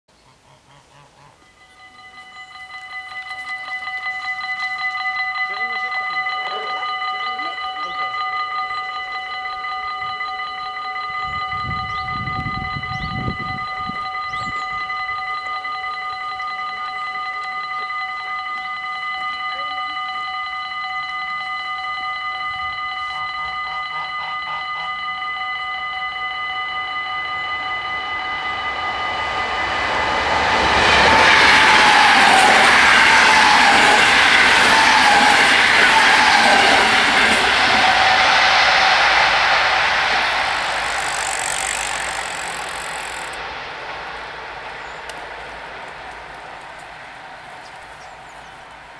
Spoorwegovergang Bergen op Zoom // Dutch railroad crossing
Locatie: Fianestraat
Traject: Roosendaal - Vlissingen
Soort: AHOB
Rode lichten: 8
Bellen: 2
Bomen: 2
Andreaskruisen: 2
Passeren:
- Mat '64 Eindhoven → Vlissingen